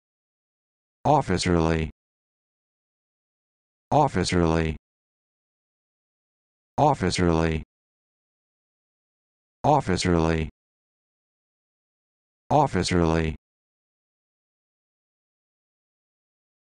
How to Pronounce Officerly
This video shows you how to pronounce Officerly